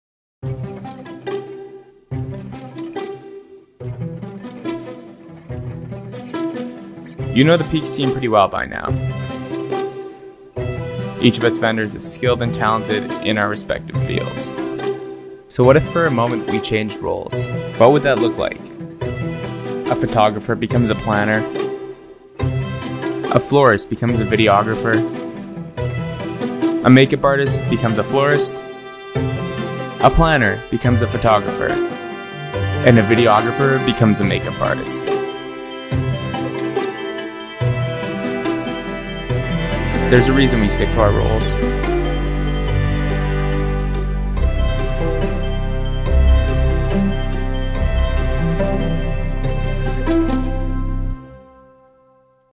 We Know Our Roles | Peaks and Vows | Official Trailer
The Peaks Team is wrapping up our first big year doing adventure and helicopter elopements. You have seen our content all year round, but what you probably haven’t seen is a lot of us. We decided to head in front of the camera, and to take on a whole new challenge of switching roles. How did we do? You be the judge.

We Know Our Roles | Peaks and Vows